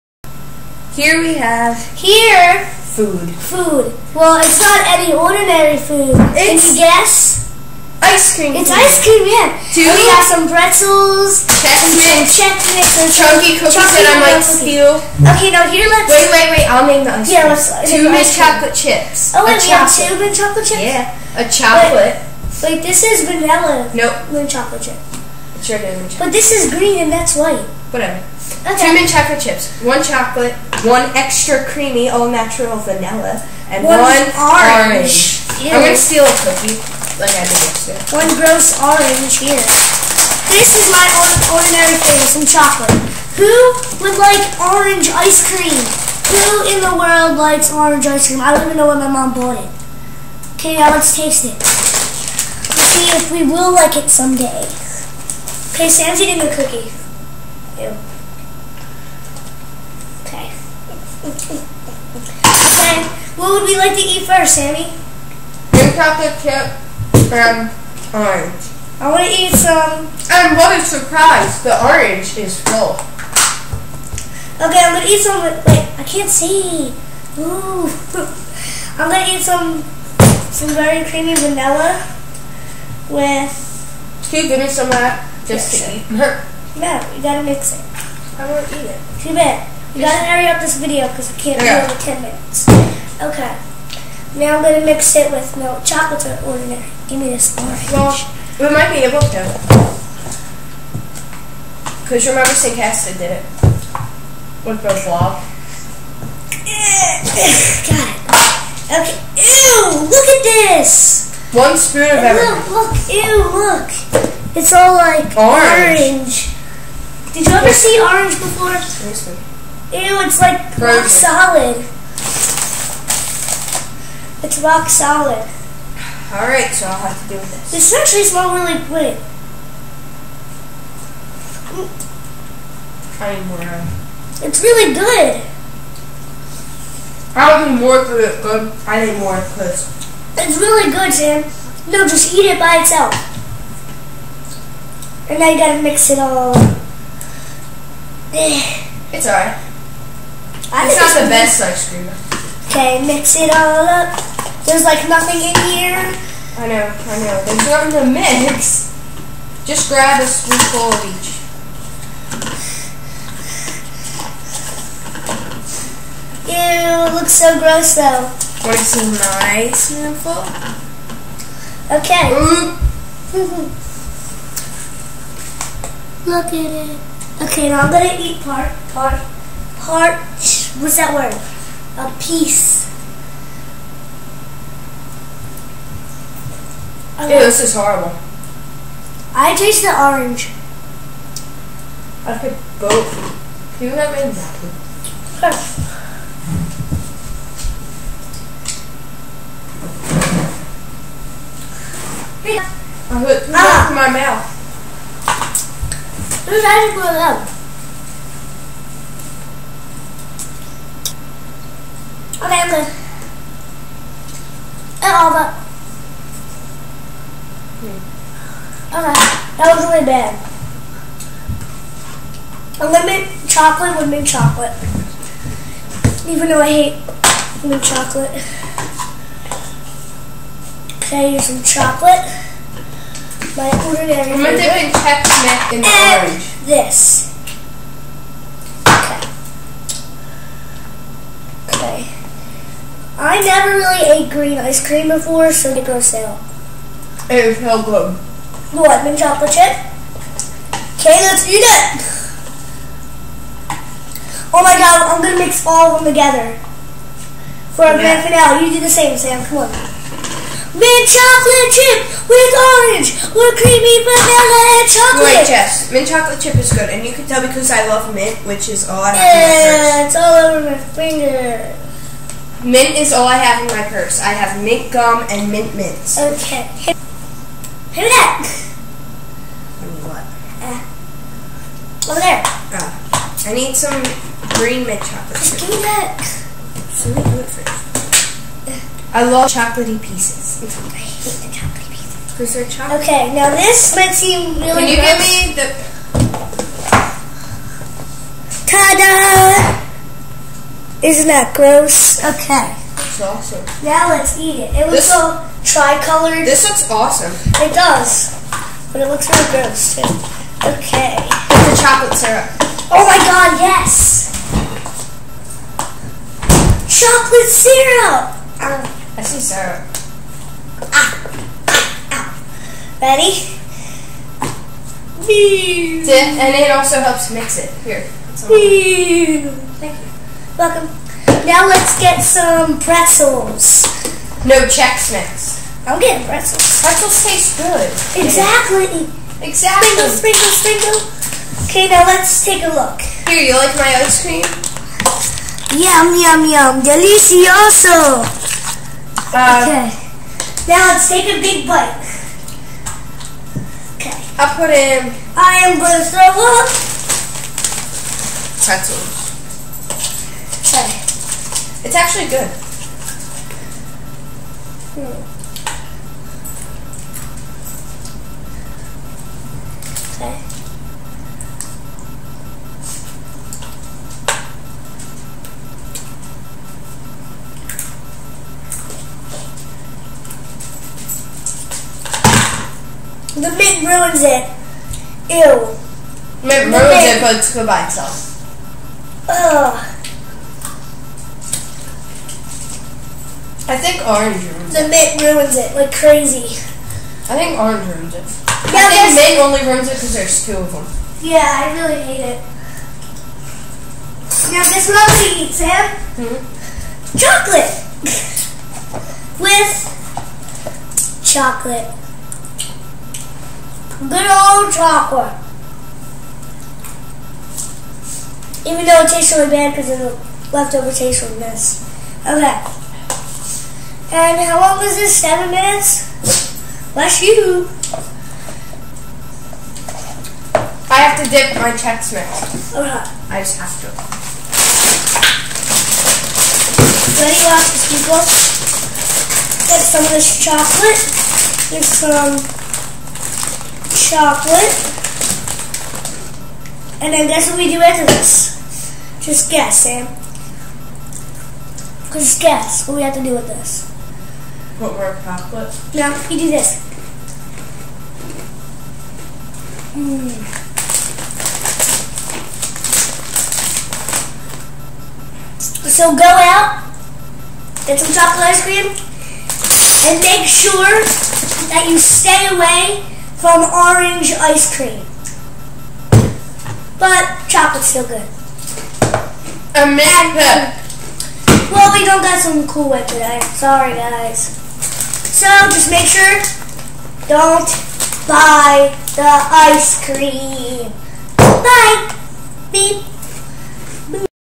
Taste Testing: Ice Cream Edition
We are taste testing again!!! Only this time our theme is all types of ice cream. But make sure you stay away from a certaing ice cream. Watch the video to find out what...